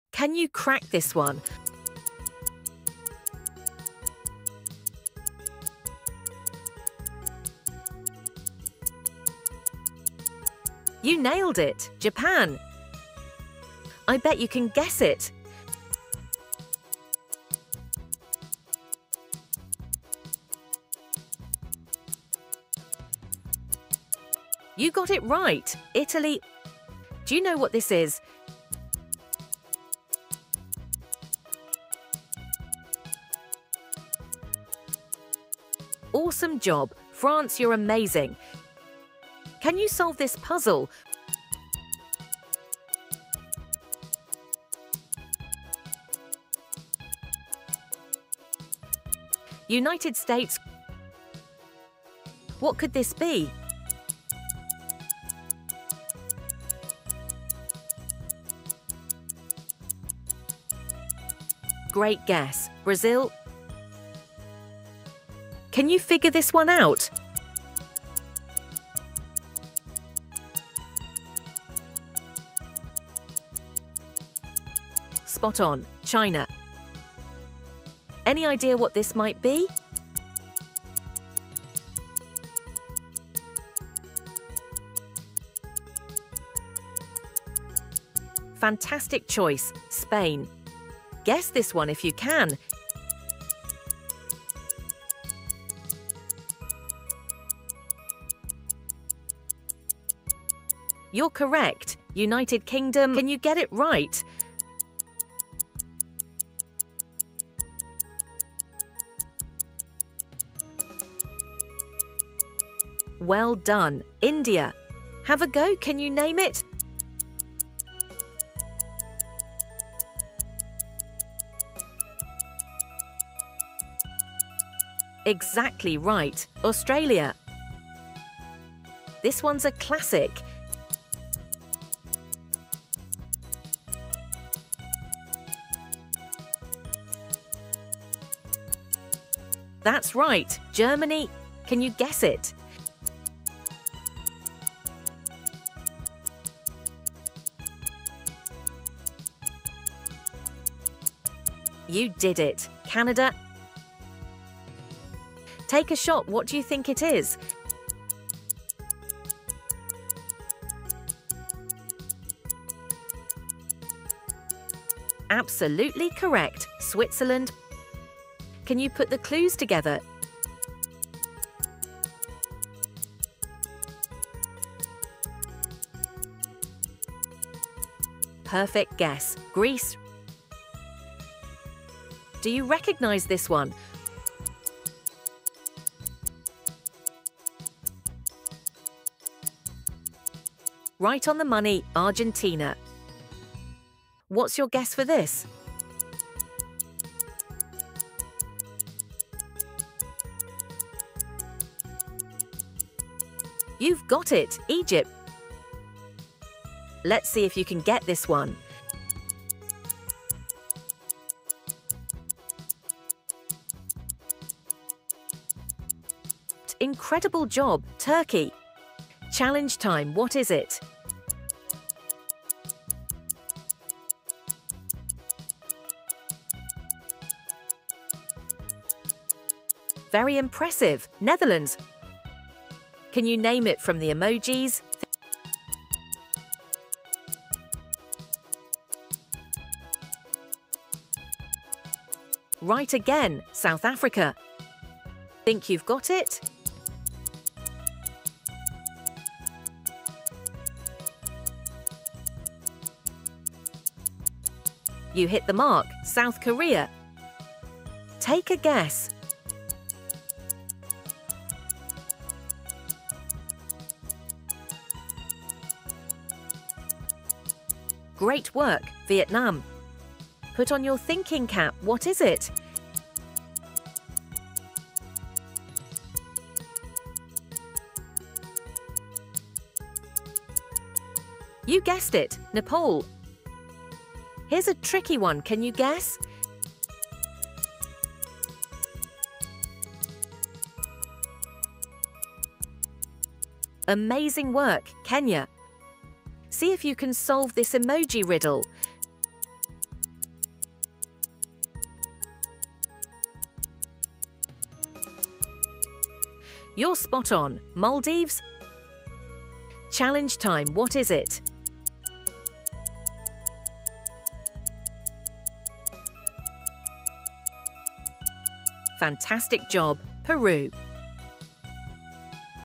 Can You Guess the Country by Emoji? 🌎🚩 | Geography Quiz Challenge
Welcome to the ultimate geography quiz challenge: "Can You Guess the Country by Emoji?" 🌎🚩 Test your knowledge and see how well you know the world! In this fun and engaging video, we present a series of emojis representing different countries. Can you guess them all before the timer runs out? Perfect for trivia lovers, geography enthusiasts, and anyone looking for a fun way to learn about countries around the globe.

Get ready to put your emoji knowledge to the test! Let's see if you can guess all the countries!